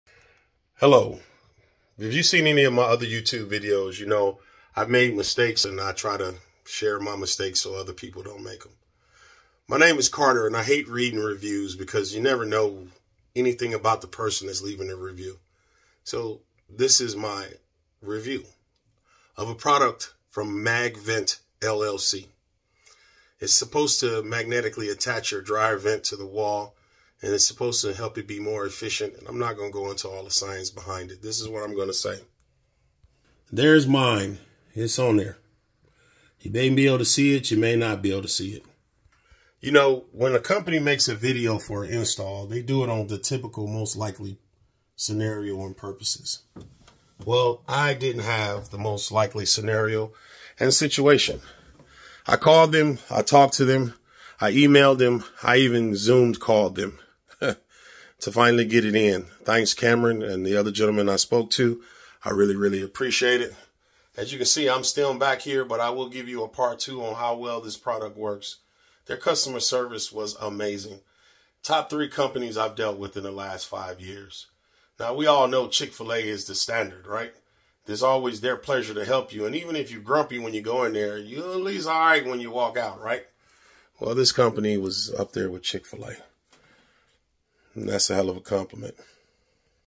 Magvent llc
Giving background on the company the customer service and the product.